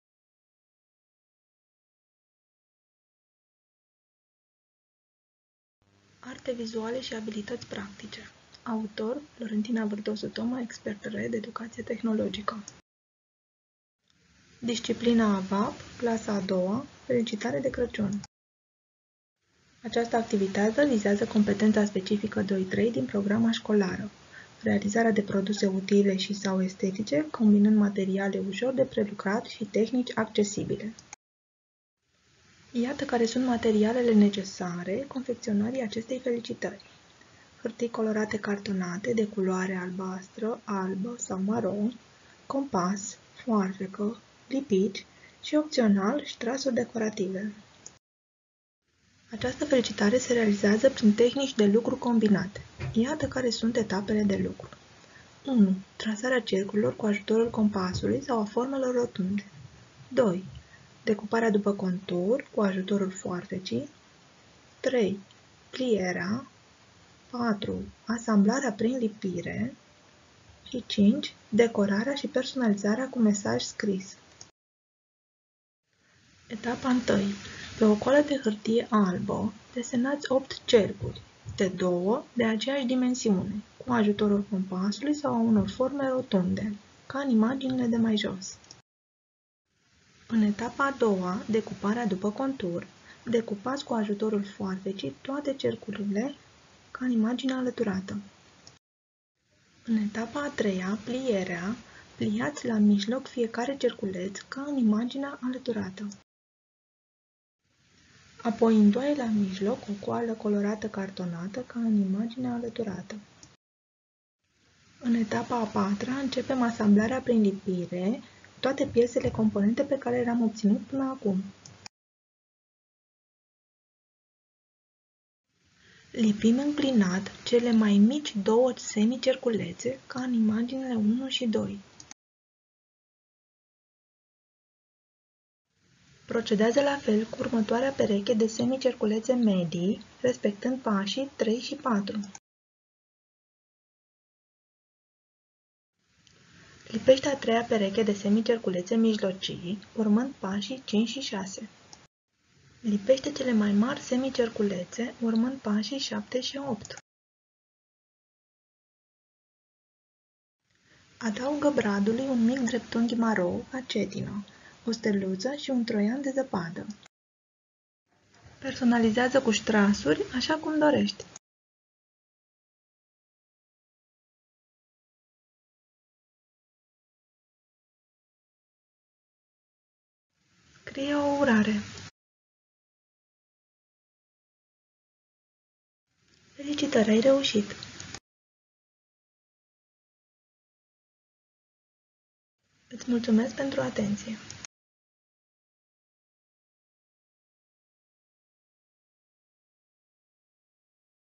Felicitare de Crăciun
Elevii realizează o felicitare de Crăciun din hârtii colorate simple și cartonate, prin tehnici de lucru combinate, astfel:
-  trasează 4 perechi de cercuri de diferite mărimi, cu ajutorul compasului sau unor forme rotunde;
-  decupează după contur cele 8 cercuri cu ajutorul cu a foarfecii;
-  pliază fiecare cerculeț după diametru, apoi le lipesc alternativ și înclinat, în ordinea crescătoare a mărimii lor, pe fața superioară a felicitării;
-  decorează cu ștrasuri  după preferințe;
-  scriu mesaje și urări în interiorul felicitării pentru destinatar.

Activitatea  se poate organiza individual și  frontal.
-   resursa poate fi folosită în procesul de predare-învățare-evaluare,  atât în activitatea la clasă, cât și în predarea online, în regim asincron și sincron.

Timpul alocat activității care utilizează resursa este de 50 de minute

Durata video-ului este de 3 minute și 11 de secunde

Autor: Florentina Vartosu Toma